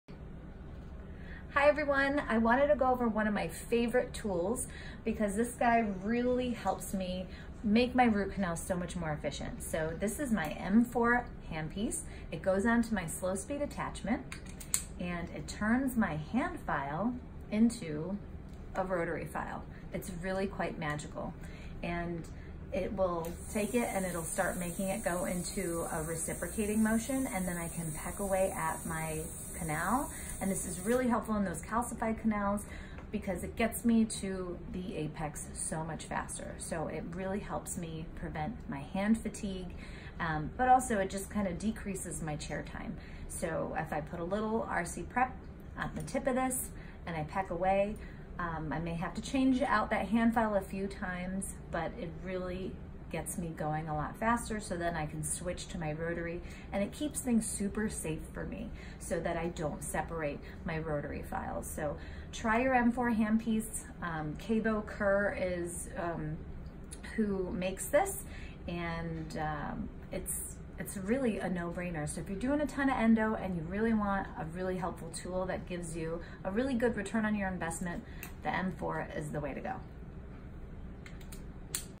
M4 Handpiece for Root Canals | Board-Certified Endodontist Dr. Sonia Chopra, DDS
I 💜 my M4 handpiece! Watch how I use this tool for more effective root canals -- it helps me negotiate curves safely, plus helps me beat hand fatigue on a calcified root.